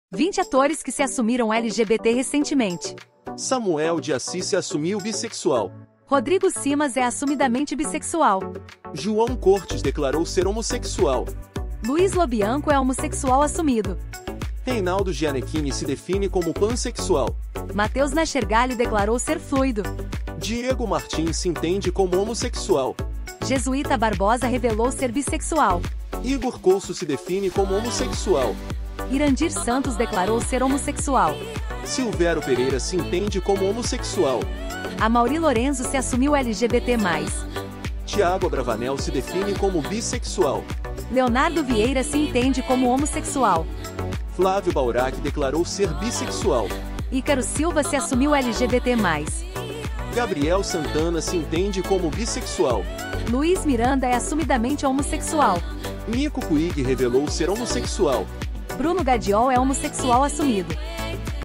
Esses 20 Atores da Globo São LGBT e Inspiram o Brasil! 👉 Veja Quais...
Esses Atores Famosos São LGBT e Inspiram o Brasil! Veja Quais...

No vídeo de hoje, apresentamos 20 atores da Globo que se assumiram LGBT e inspiram o Brasil com suas histórias de coragem e autenticidade! Esses artistas brilham nas telinhas e representam a comunidade LGBT+ de maneira poderosa.

Entre os destaques, temos Rodrigo Simas, Irandhir Santos e Matheus Nachtergaele, todos do elenco da novela Renascer, que marcaram suas trajetórias na TV com grande talento. Na novela Terra e Paixão, os atores Amaury Lorenzo (Kelvin) e Diego Martins (Ramiro) também conquistaram o público com suas atuações e representatividade.

Veteranos como Reynaldo Gianecchini e Rainer Cadete continuam a brilhar, mostrando que é possível ser autêntico e bem-sucedido ao mesmo tempo. A presença de todos esses atores em novelas como Renascer e Terra e Paixão fortalece a diversidade e amplia a visibilidade LGBT+ nas telinhas.